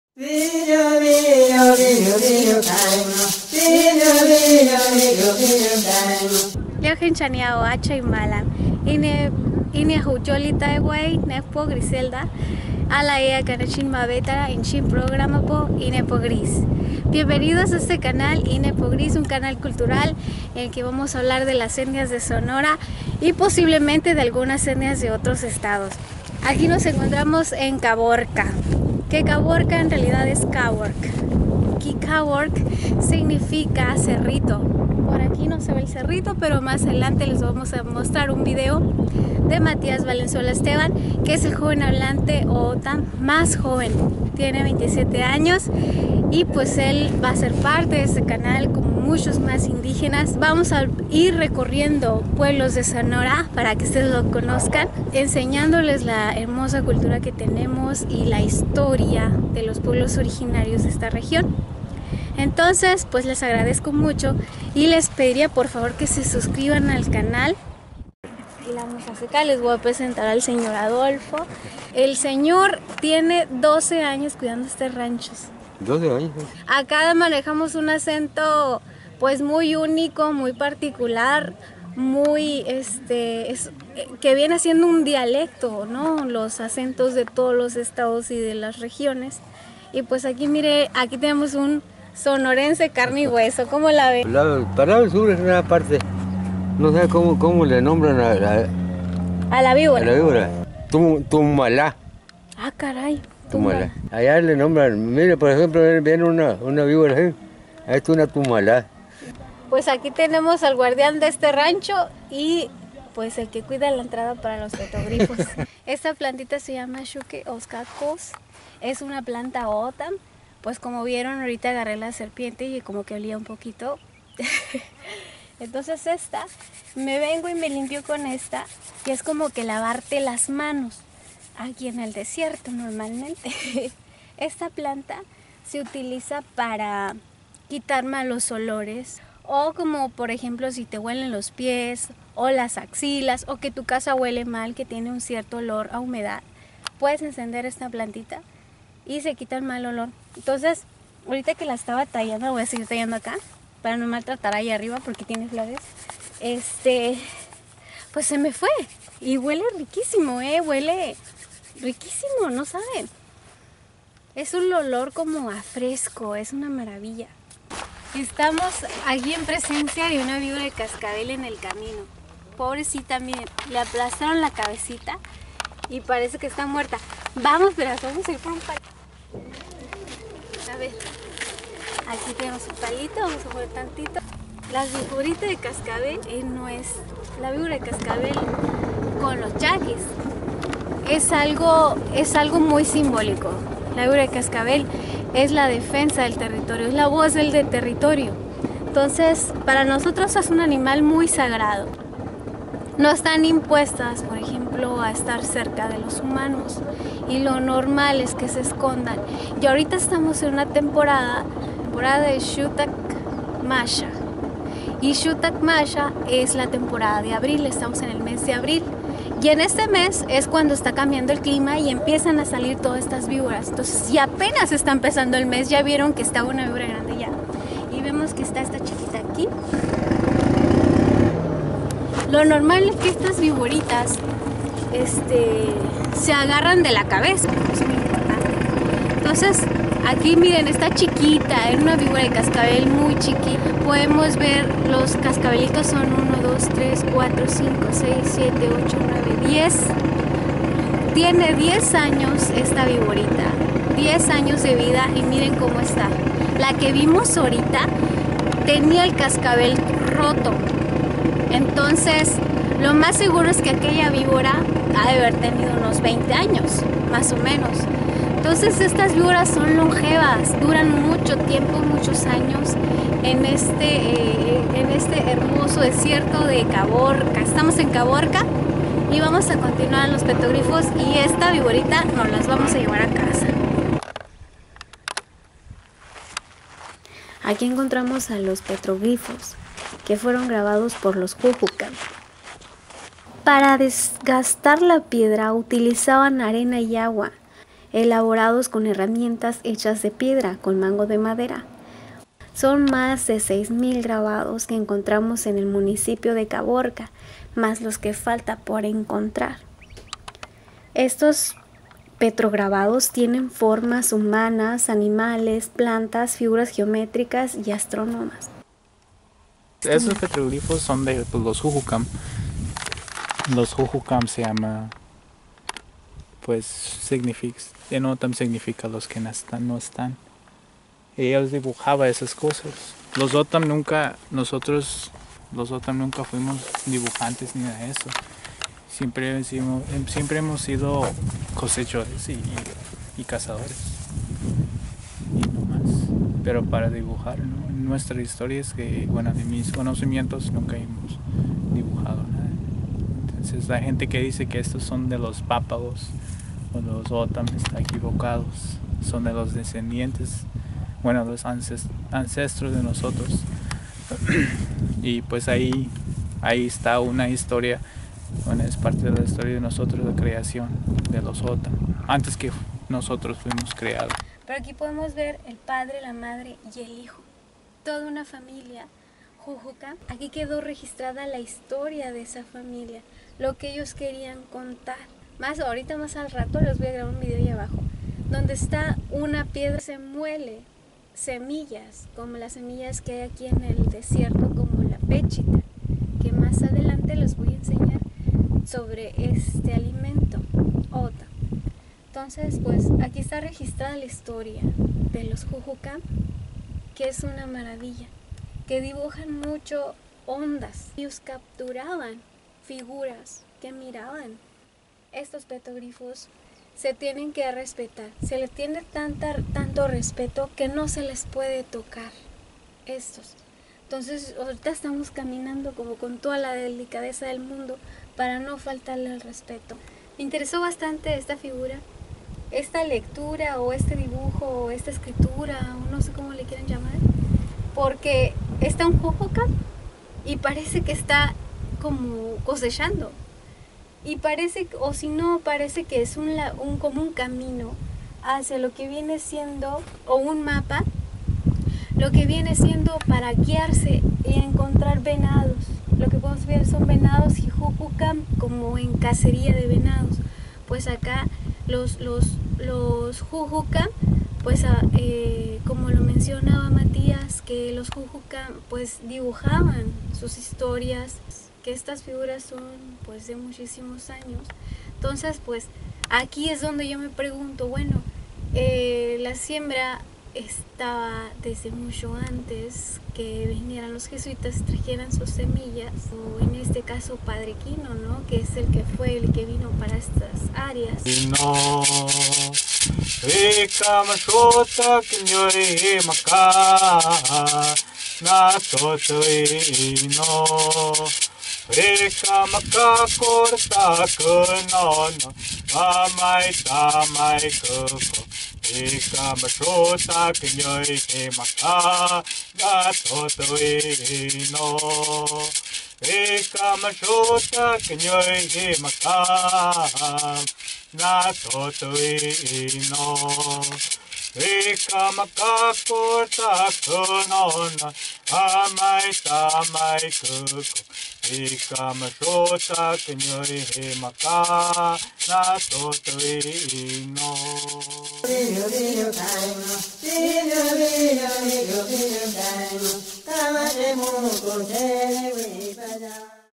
Petrograbados de Caborca Sonora.
programa cultural
la conductora indígena Griselda Renteria Alvarez yaqui y tohono o'otham les presenta una pequeña parte de los petroglifos de los ancestros o'otham.